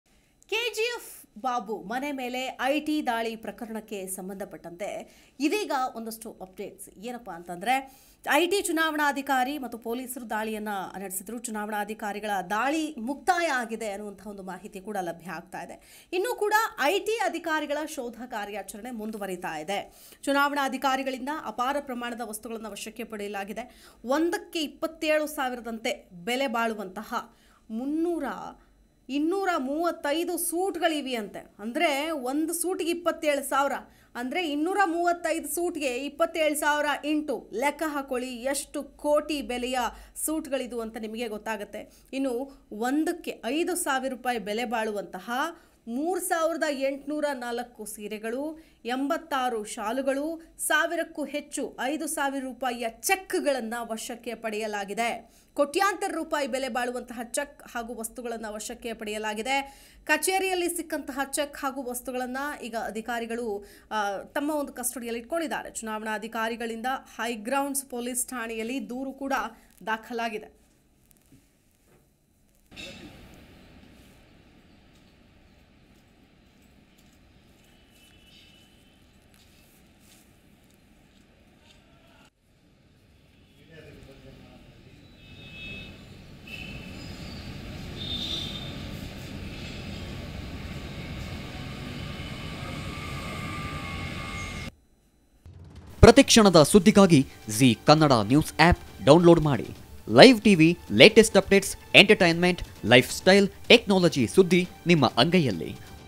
KGF Babu | ಕೆಜಿಎಫ್ ಬಾಬು ಮನೆ ಮೇಲೆ ಐಟಿ ದಾಳಿ.. ಕೋಟಿ ಮೌಲ್ಯದ ವಸ್ತು ವಶ!
ಕೆಜಿಎಫ್ ಬಾಬು ಮನೆ ಮೇಲೆ ಐಟಿ ದಾಳಿ ಪ್ರಕರಣ.ಐಟಿ, ಚುನಾವಣಾಧಿಕಾರಿ, ಪೊಲೀಸರಿಂದ ದಾಳಿ. ಚುನಾವಣಾಧಿಕಾರಿಗಳ ದಾಳಿ ಮುಕ್ತಾಯ.